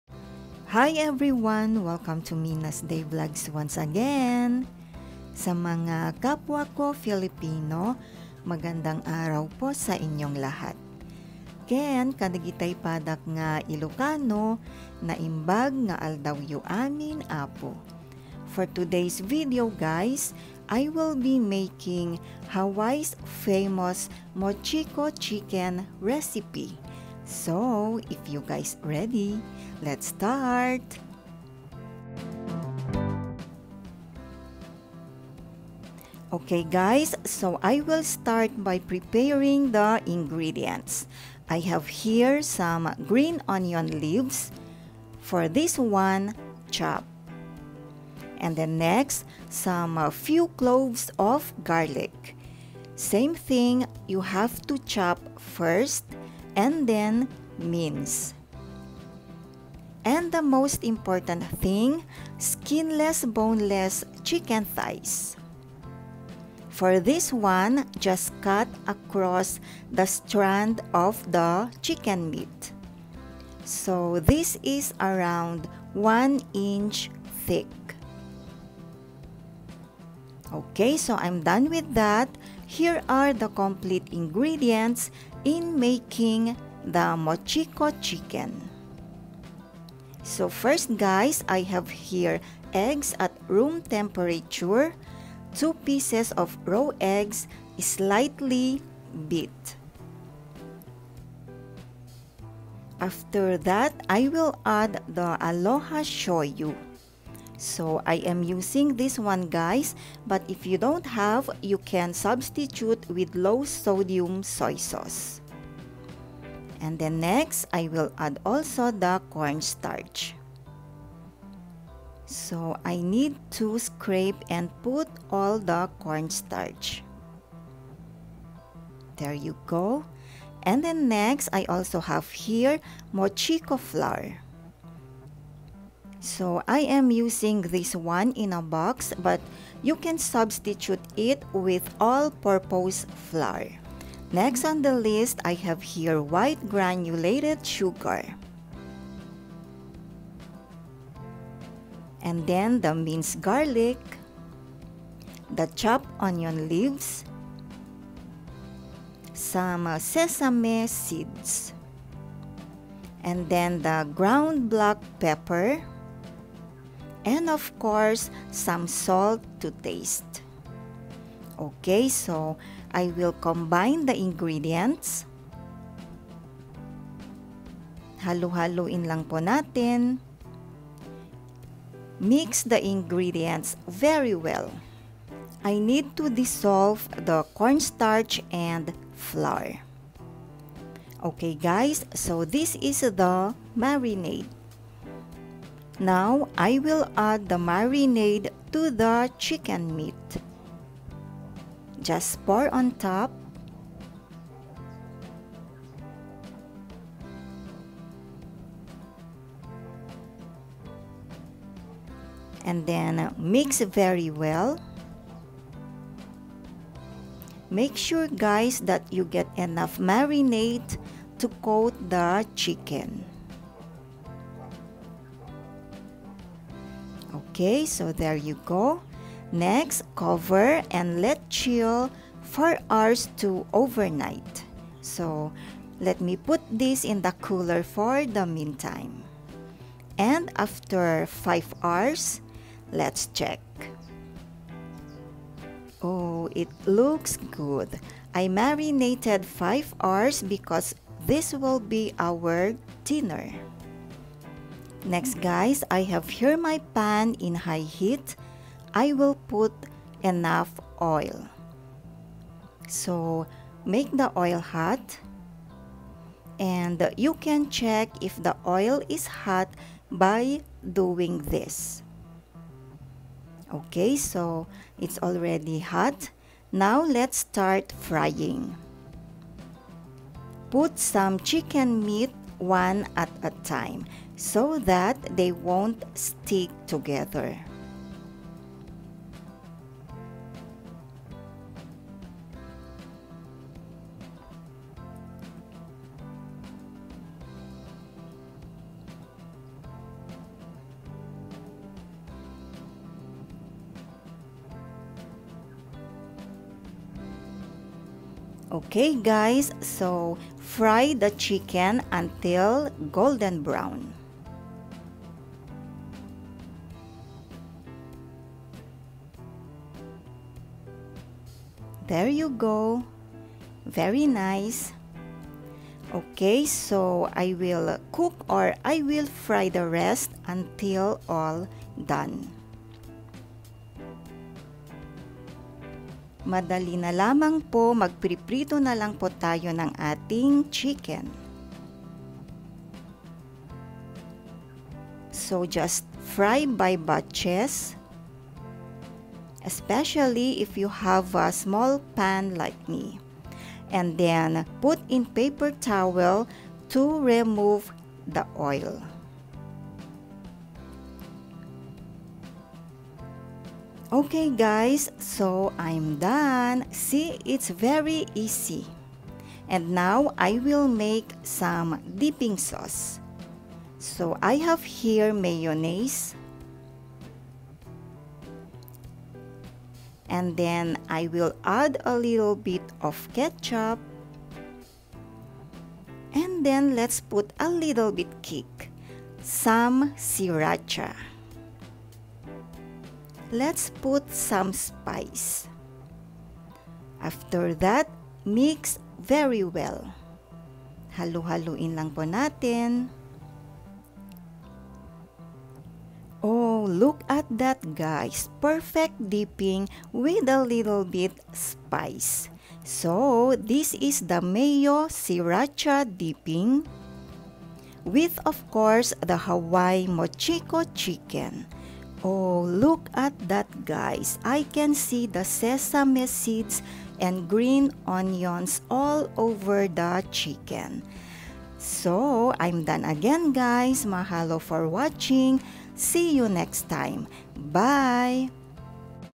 Mochiko Chicken | Hawaii Style Mochiko Chicken | How i make Mochiko Chicken plus Spicy Dipping
Mochiko Chicken is a very popular dish/recipe in Hawaii, Chicken thighs  thinly sliced and marinated at least 4 hours to overnight then fry afterwards. I also made Mayo Sriracha dipping to make it more exciting.

Lists of Ingredients in the video.
Enjoy watching my friends and Happy Cooking!

Let’s make Ono Mochiko Chicken.

Mahalo,
Mina’s day Vlogs